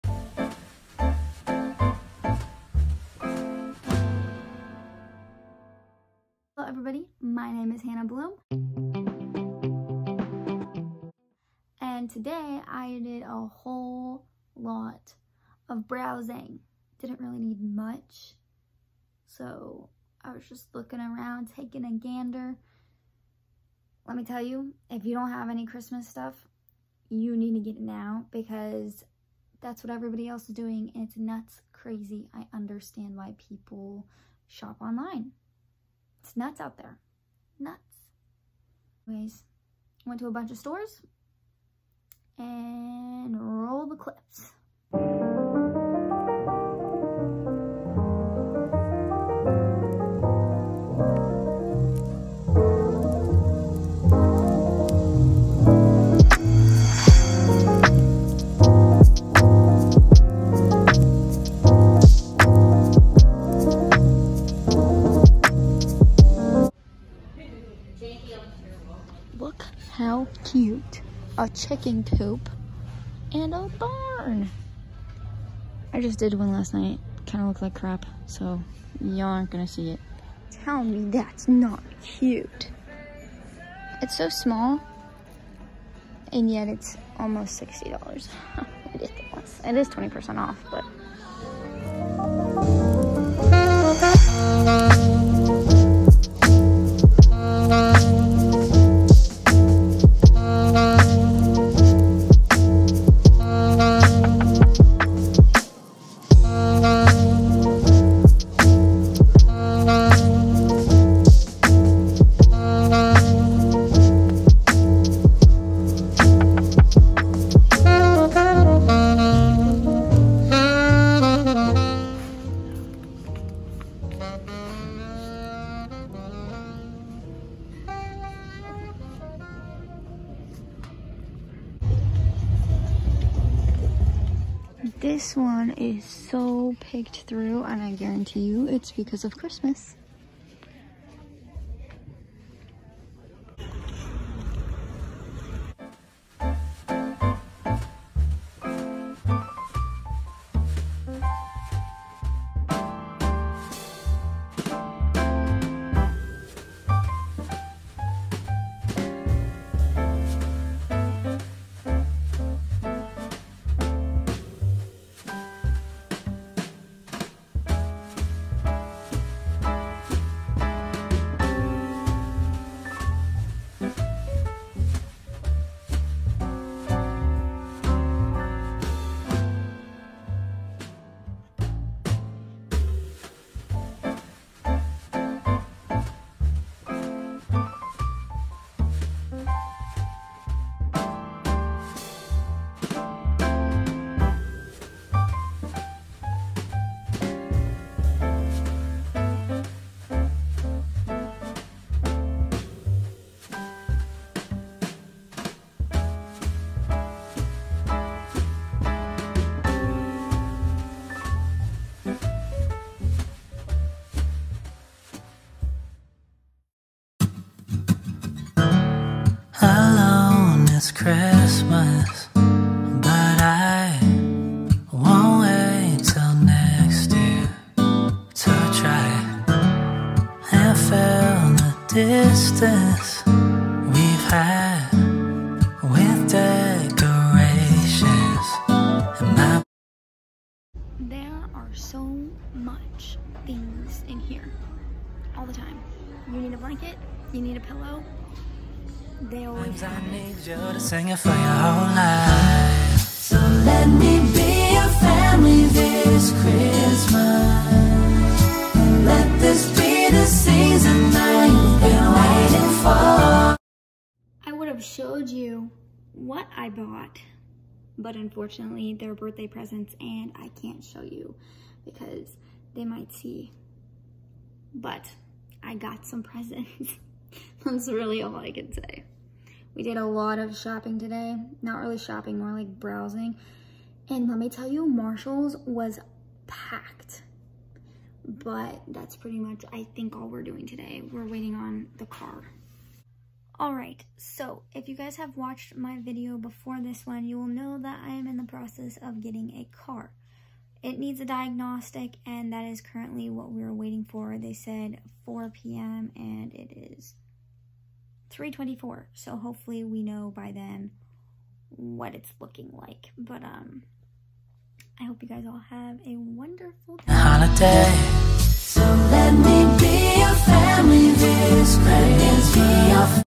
Christmas shopping!|Marshalls,Dollar tree,Tractor Supply + more
I hope you’ve enjoyed this video and get on top of all your Christmas shopping!
There as so many cute gift sets out there to make it easier on you.

Music I used:

The rest of the music is not my own.